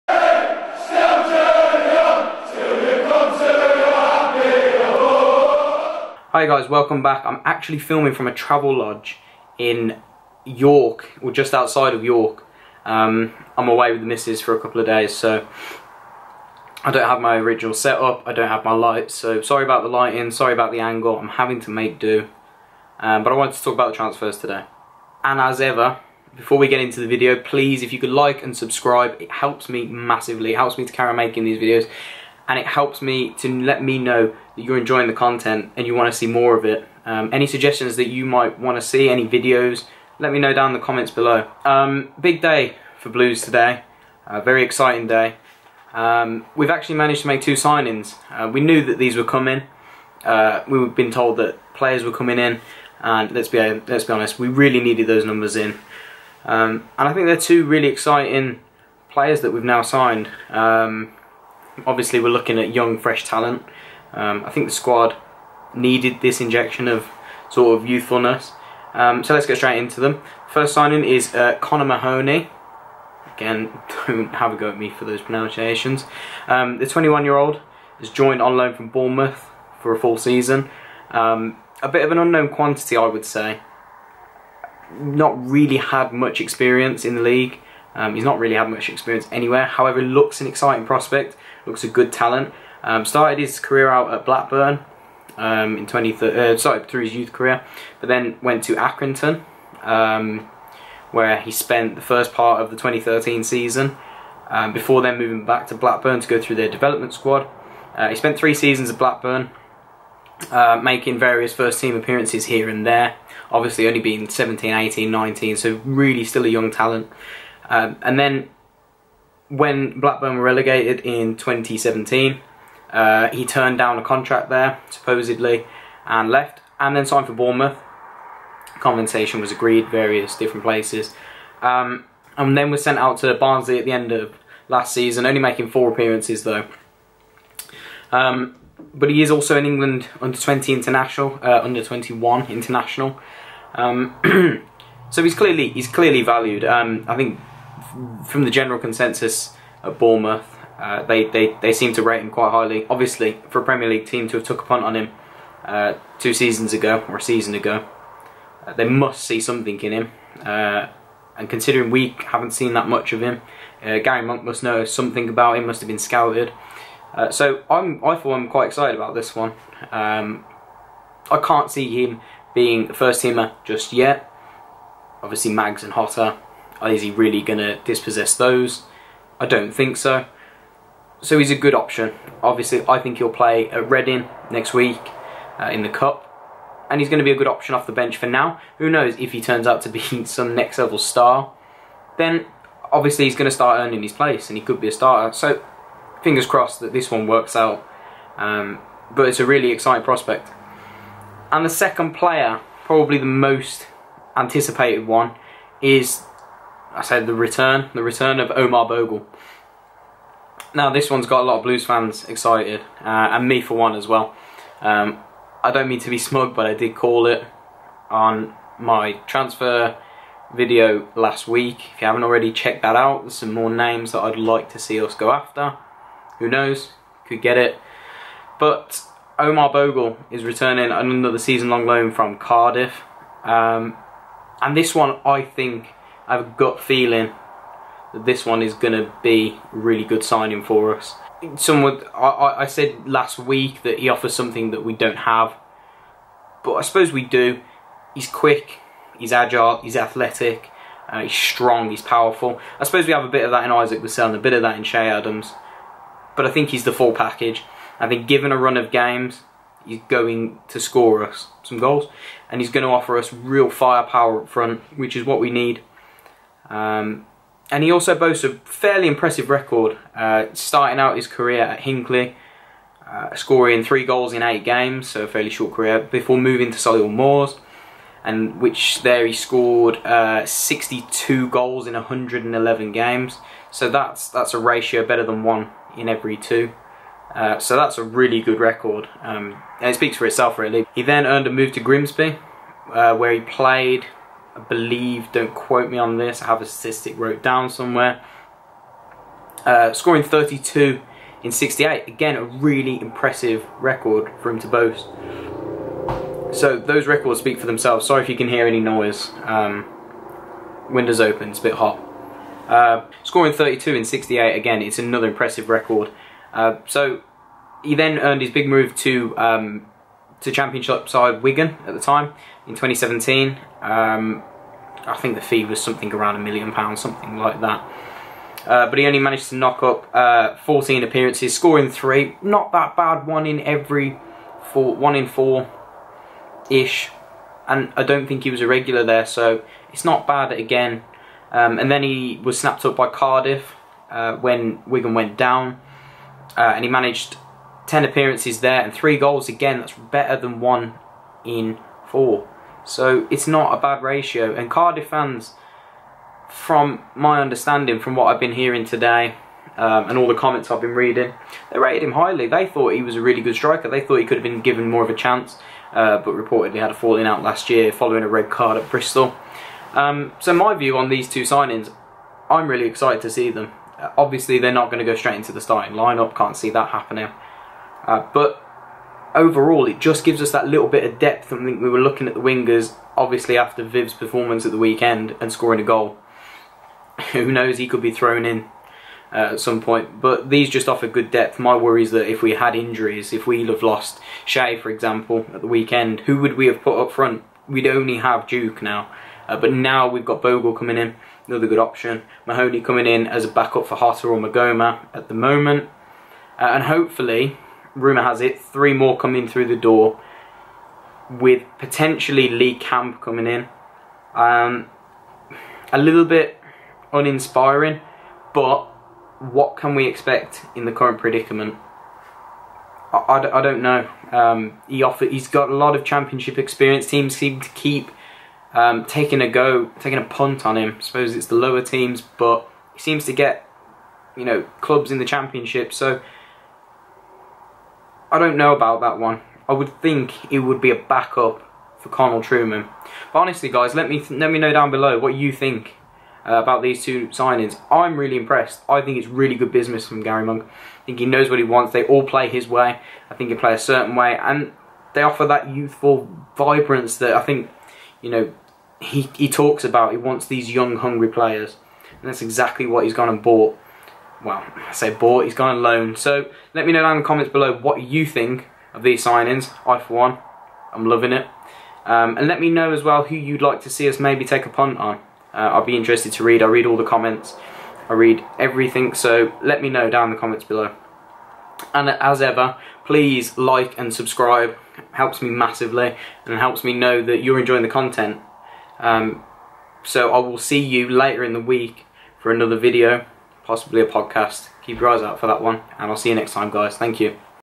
Blues double swoop!! - Transfer talk Omar Bogle and Connor Mahoney join Blues
Blues double swoop!!

Transfer talk Omar Bogle and Connor Mahoney join Blues. talking about about Blues' double swoop for the young duo.